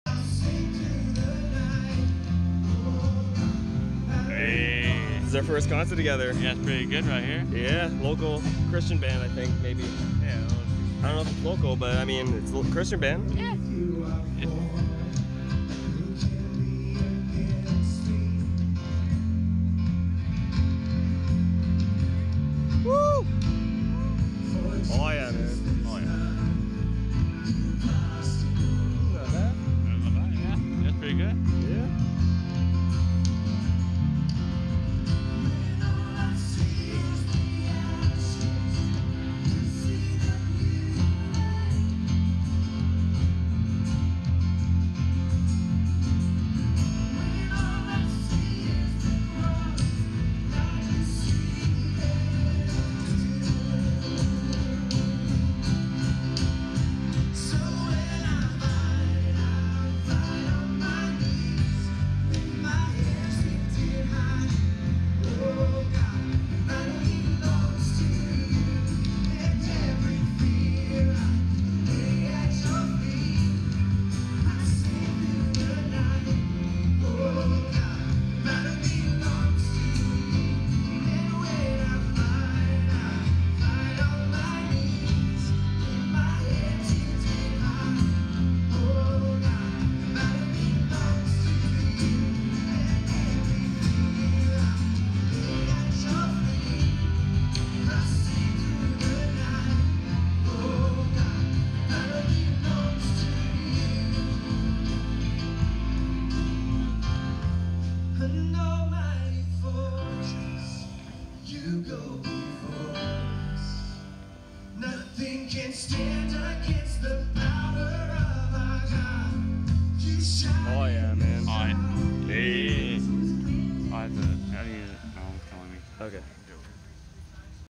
Christian band concert at Tecumseh Park
There is a Christian band playing at Tecumseh Park in Chatham today, they're pretty good.